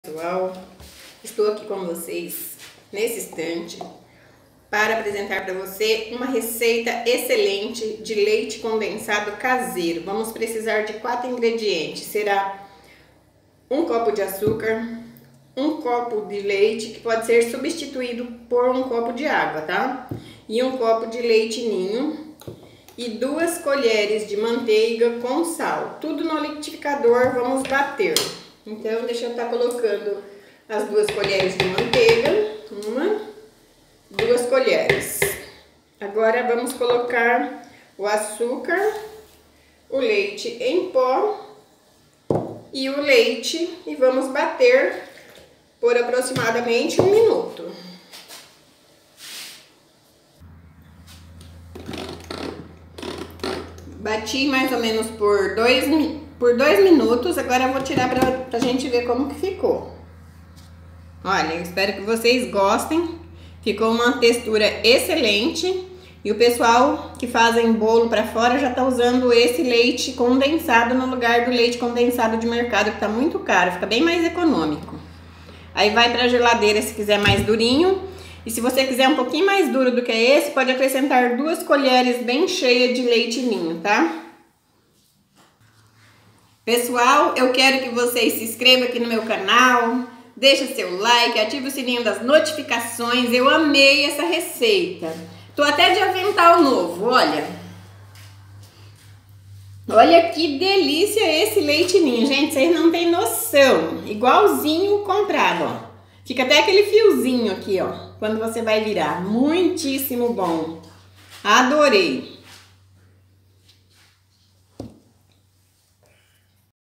LEITE CONDENSADO CASEIRO !!!
Esta receita é muito fácil , quatro ingredientes. delicioso leite condensado , você não vai se arrepender.
1 copo de leite ninho
1 copo de leite ou água.
1 copo de Açúcar .
2 colheres de manteiga com sal.
pessoal coloca o copo de leite ninho bem cheio ai não precisa acrescentar as duas colheres porque quando leva a geladeira ele exelente a consistência muito obrigado espero que gostem ❤❤❤❤